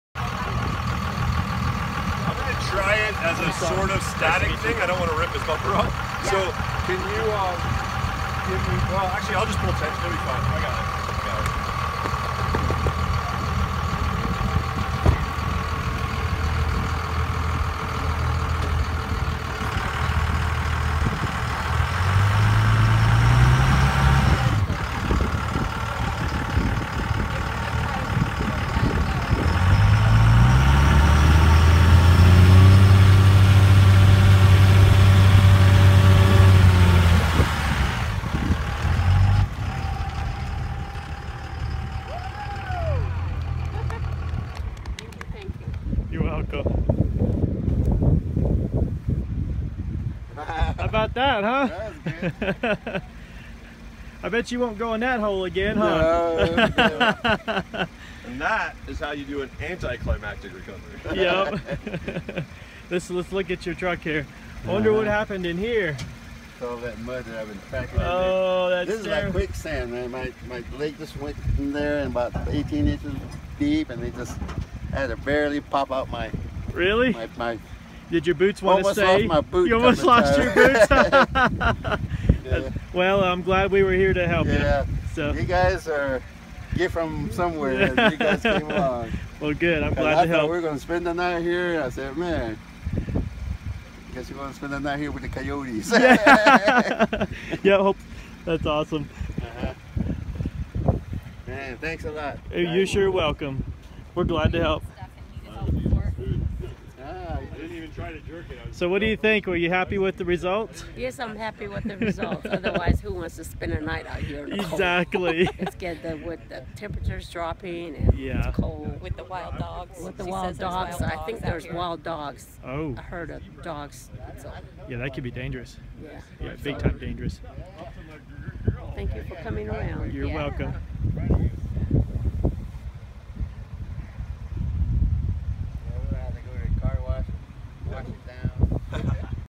Exploring Elements rescues stuck pickup at the Little Colorado River
Bryon Dorr from Exploring Elements pulls a very stuck truck out of the mud, we got to interview them and they were very happy we happened by.